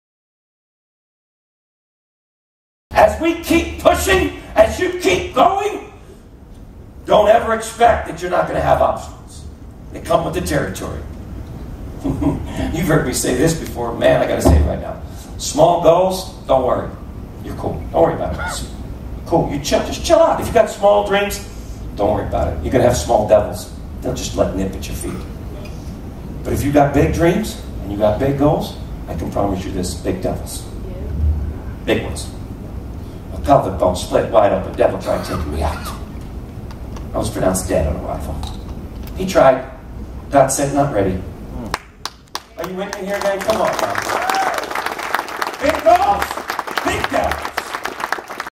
"Big Dreams, Big Goals, Big Devils I can promise you that."
On your path you will have obstacles... as Eric Giglione says, "Big Dreams, Big Goals, Big Devils I can promise you that."